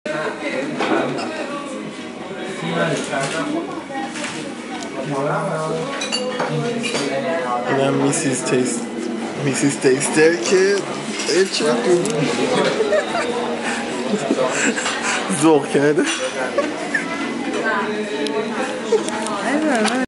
رستوران باکس و دسر
میسیز تیستر در رستوران باکس در حال تیست دسر و موس شکلات. رستوران باکس در دیباجی شمالی